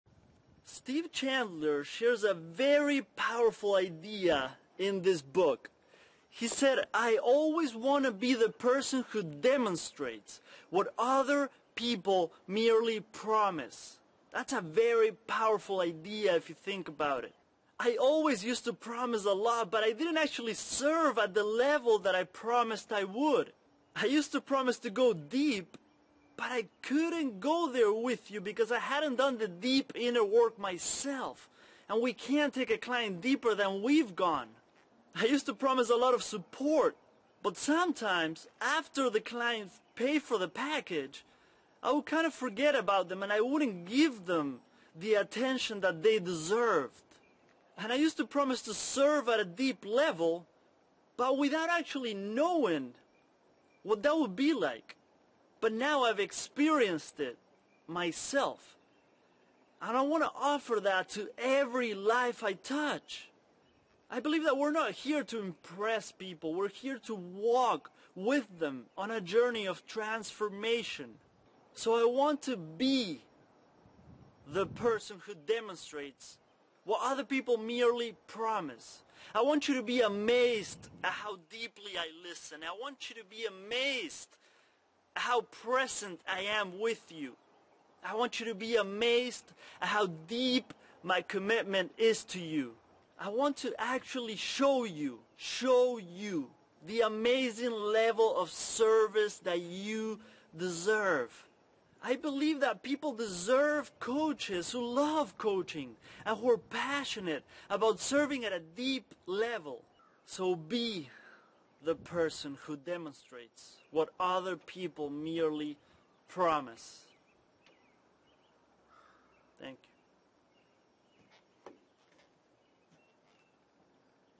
The Prosperous Coach: DEMONSTRATE What Others Merely Promise - Gonzalo Corts
Get 2 FREE AUDIBLE BOOKS!

In the book The Prosperous Coach, Steve Chandler shares a powerful idea.

He says “I always want to be the person who demonstrates what other people merely promise.”

That’s a very powerful coaching tip if you think about it.

I used to promise many things but didn’t actually serve my clients at the level I promised I would.

In all of my jobs, as a Personal Trainer, Dietitian, Holistic Health Coach, Life Coach, and others, I ALWAYS used to promise more than I delivered.

Unintentionally, but it happened.

I am now at a place in my journey where I truly want to DEMONSTRATE what I used to merely promise.

I now SHOW my clients how deeply I listen, how present I am with them, how deep my commitment is to serving them, and more.

People DESERVE amazing service. We all do.

People DESERVE coaches who LOVE coaching and are passionate about serving.

So… my commitment has been…

To BE the person who demonstrates what others merely promise.

GonzaloCortsCoaching